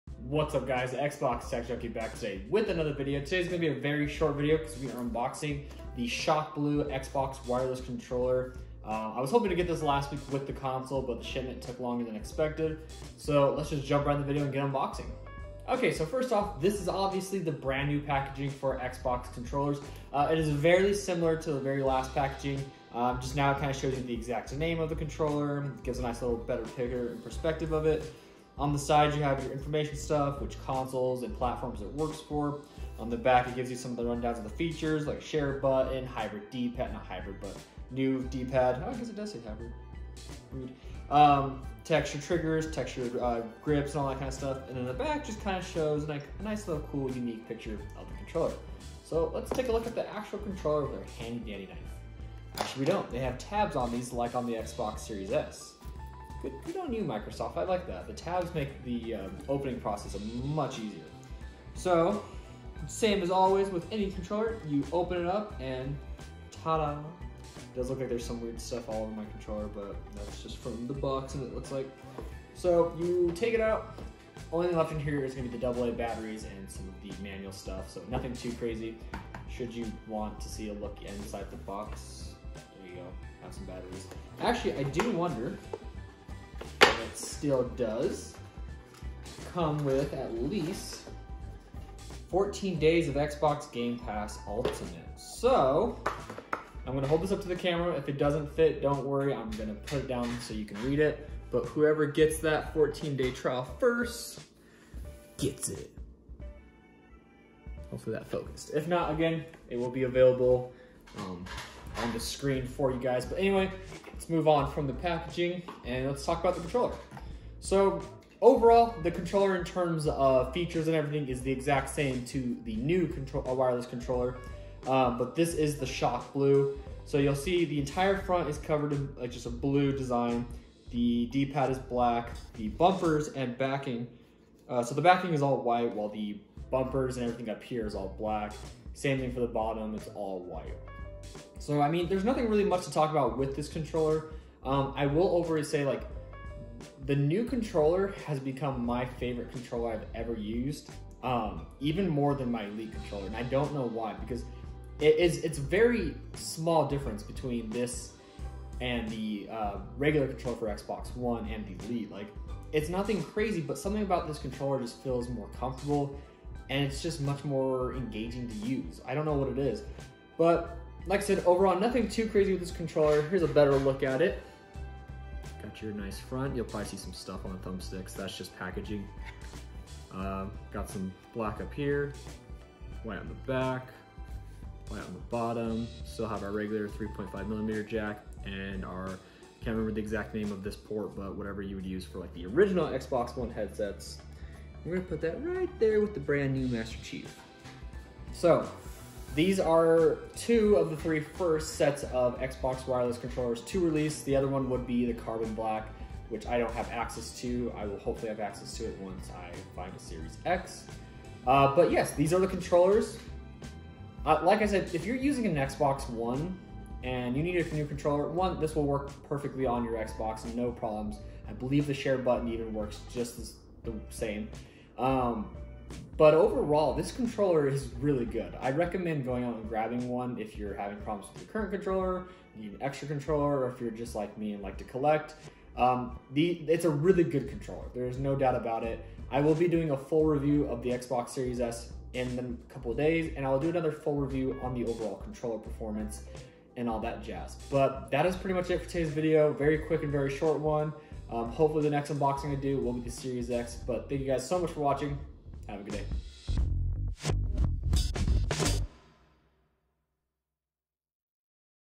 Unboxing The Shock Blue Controller
Today we will be unboxing the all new Wireless Xbox Shock Blue Controller!